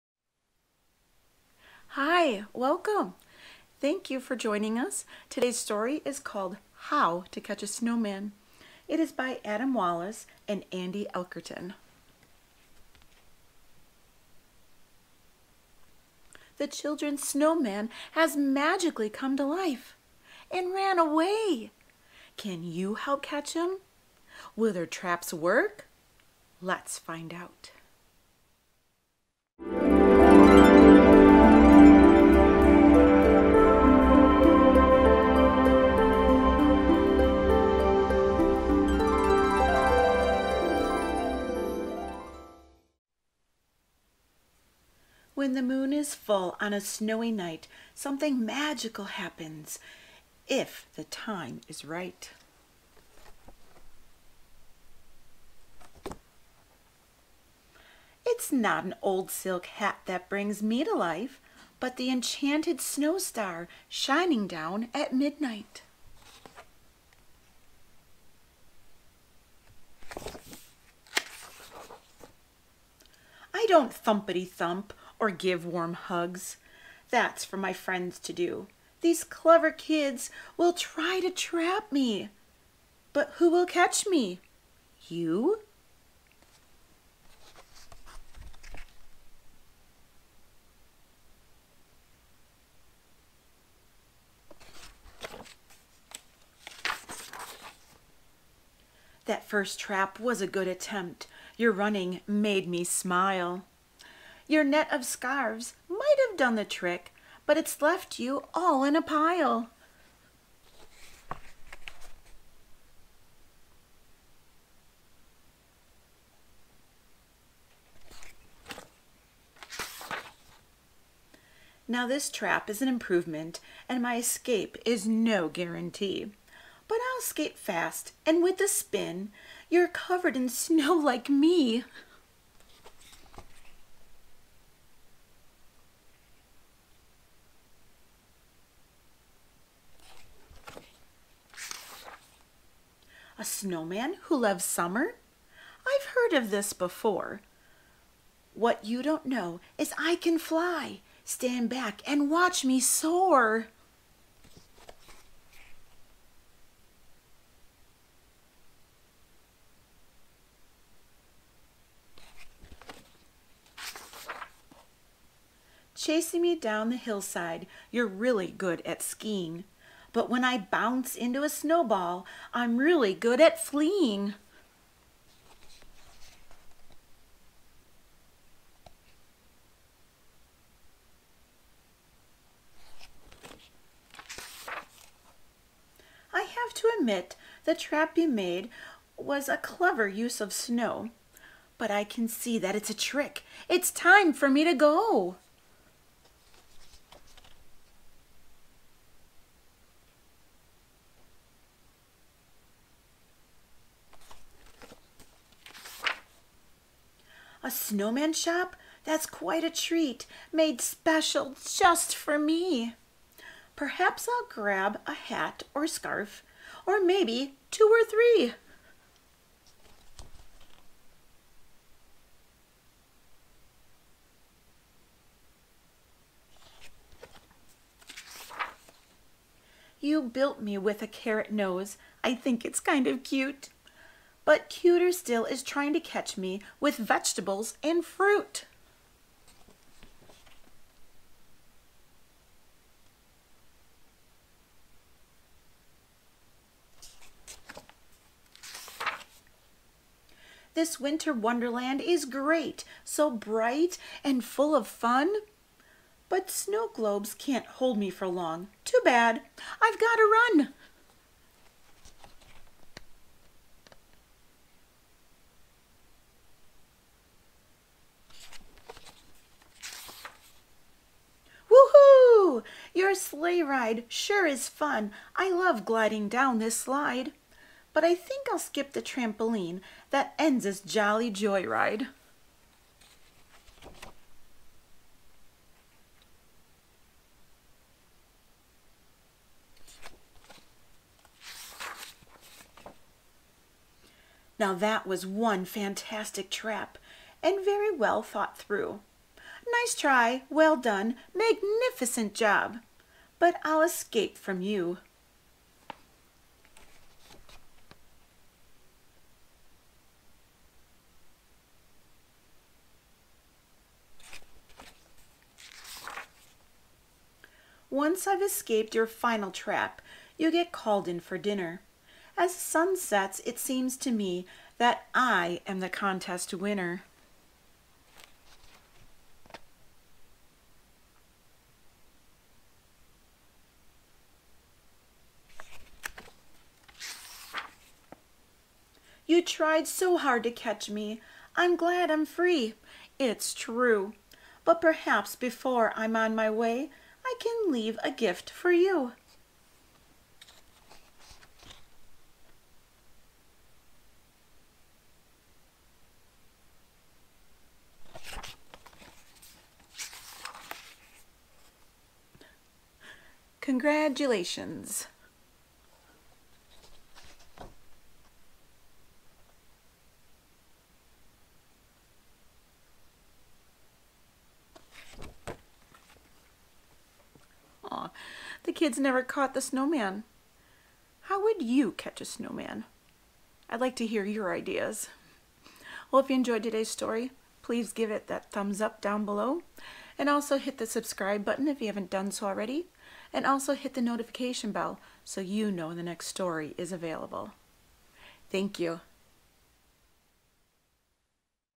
How to Catch a Snowman
Miss Wendy reads "How to Catch a Snowman" by Adam Wallace and Andy Elkerton

or on Scholastic.